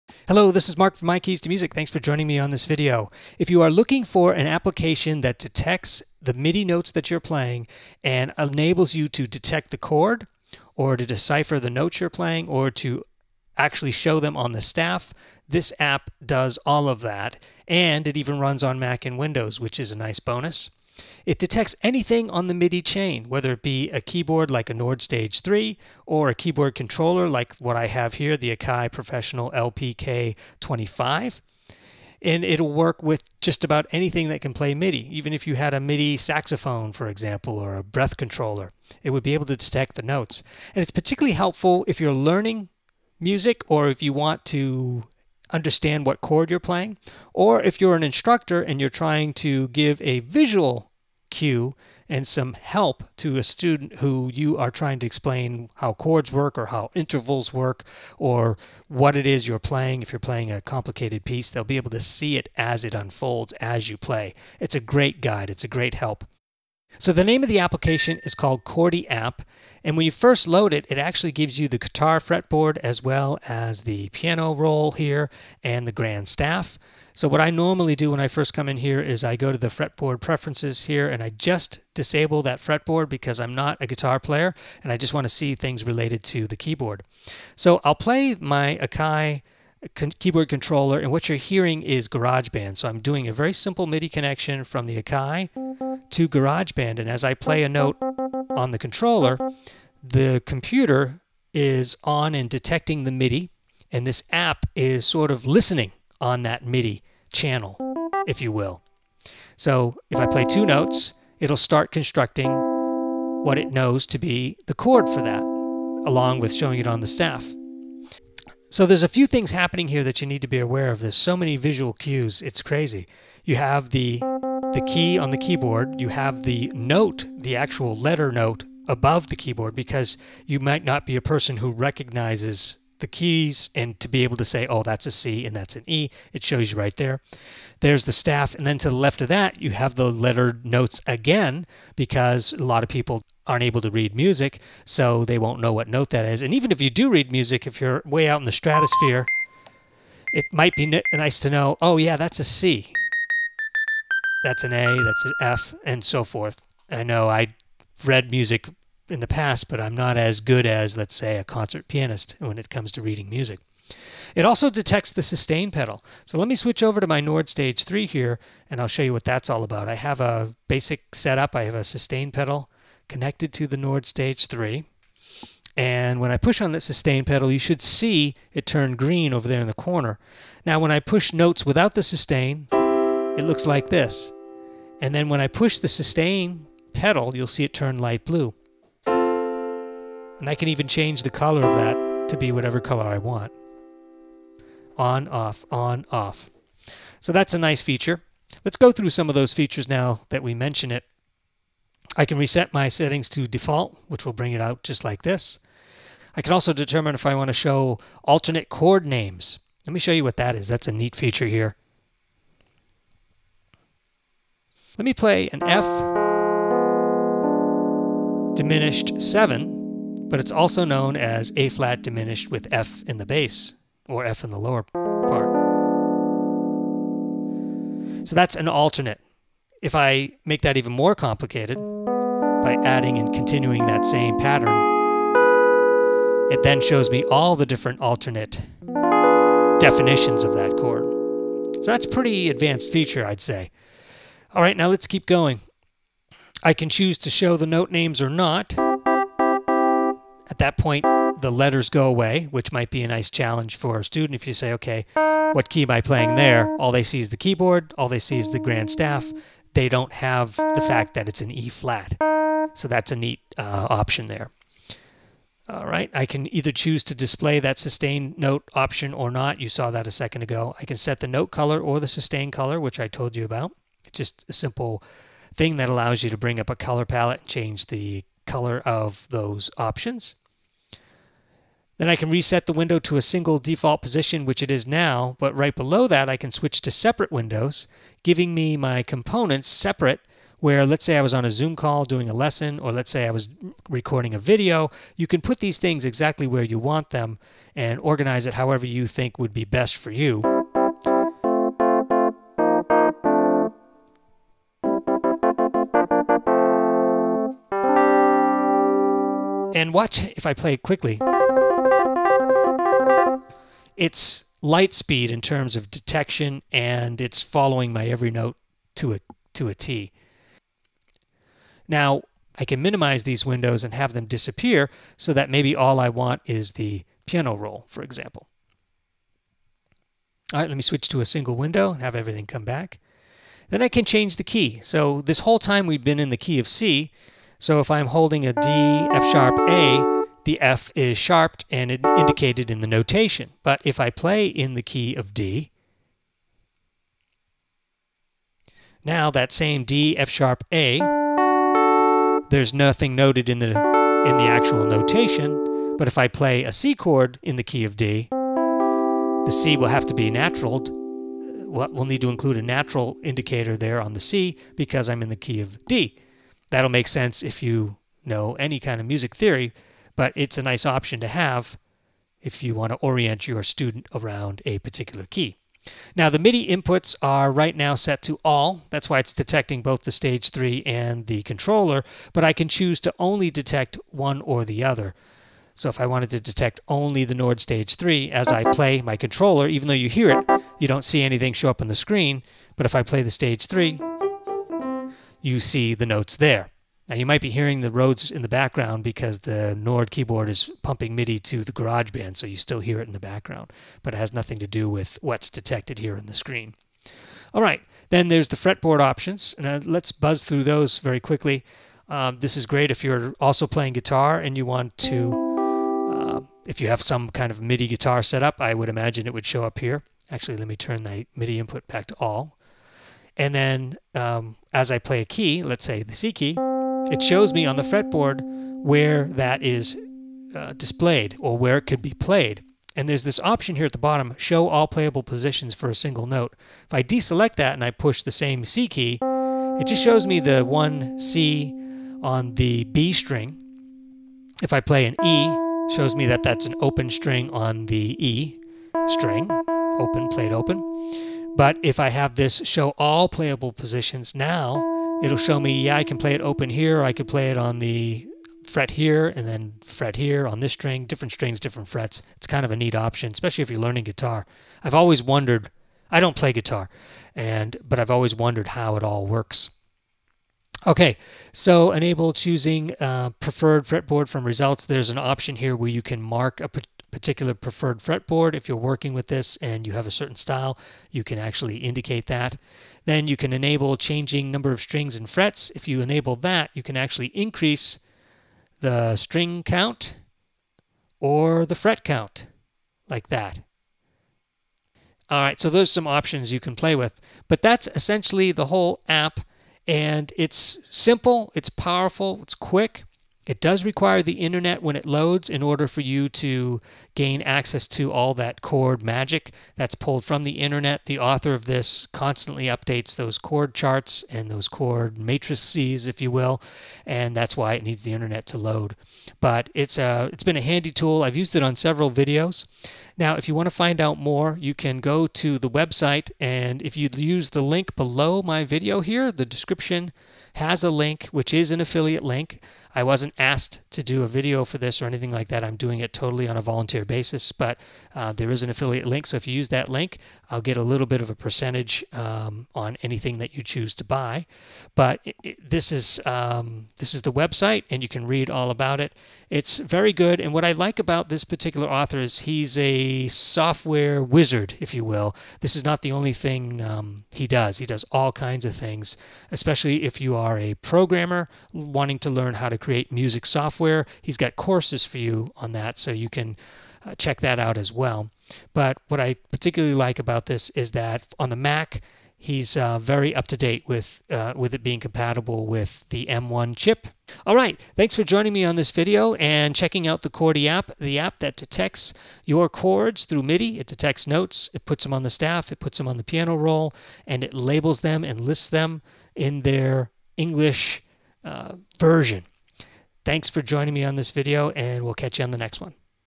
A Solution to Detect Chords / Notes Via MIDI (ChordieApp Review and Tutorial)
🙏🏽 Thanks for all the support and for the shared love of music. 🎶

In this video, I review a computer application called “ChordieApp” that detects notes and chords and displays them in a variety of ways including placement on the grand staff. This is a good solution if you are learning chords or you are an instructor and want people to see the exact notes/chords you are playing as you play them.

The app is MIDI compatible and can analyze any MIDI device that is connected to your computer.  You can position the windows anywhere on the screen which makes it great for live presentations or to make your own videos.  It’s the perfect way to demonstrate your performance especially if your goal is to help guide people with the exact notes/chords you are playing.

00:00 - Intro to ChordieApp
01:05 - ChordieApp Feature
03:18 - ChordieApp Options and Preferences
05:33 - Set the key in ChordieApp
06:47 - Fretboard Options for ChordieApp
08:17 - Learn More or Purchase ChordieApp

👀 Buy your Nord keyboards at Sweetwater and get a 2-year warranty! (U.S. Customers)